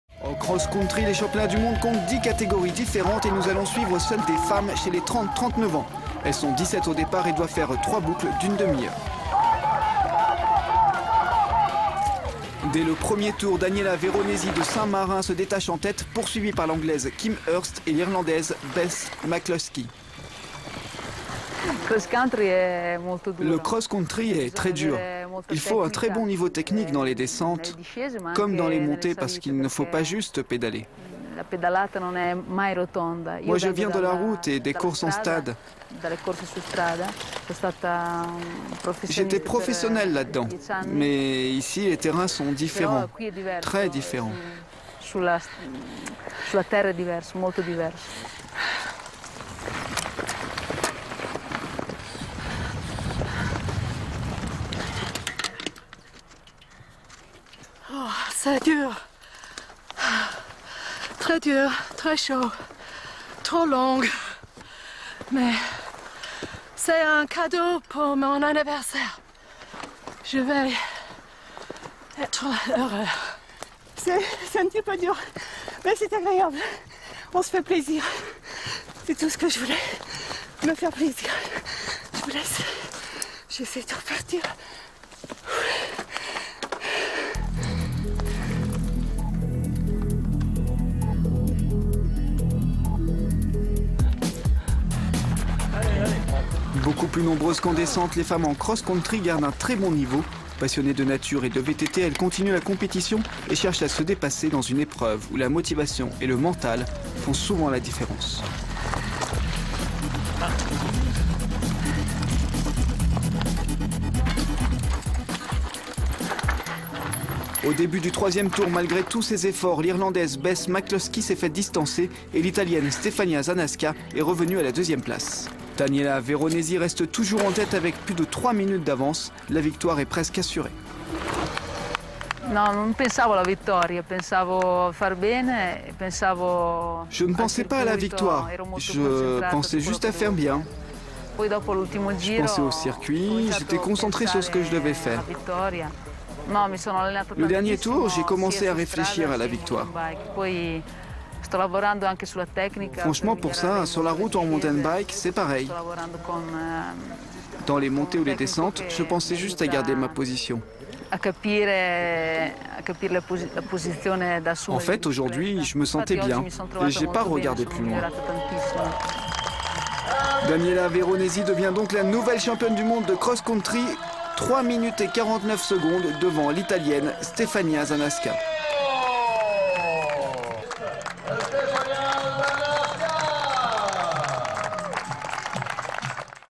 MountainBike World Championship UCI Master - Praloup 2009
Edith au championnat du monde masters de VTT 2009, à Praloup